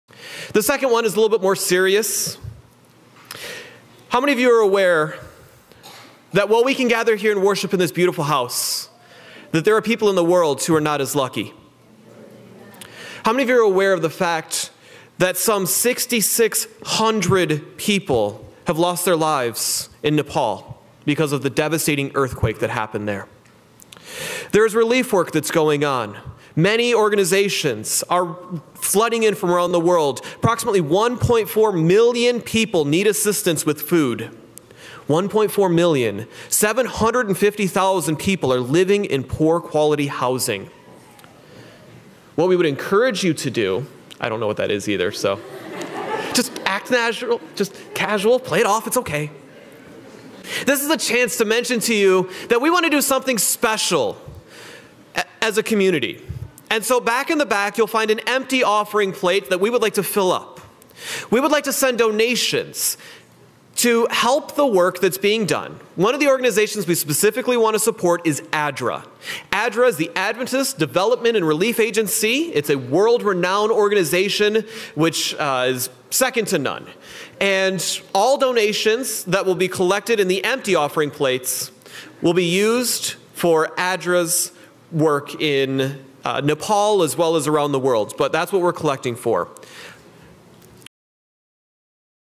Earthquake During Earthquake Relief Appeal
Did you feel the earthquake today? We sure did—right when Pastor Taylor gave his appeal for Nepal earthquake relief through ADRA! Excellent timing. The shaking starts about 0:25.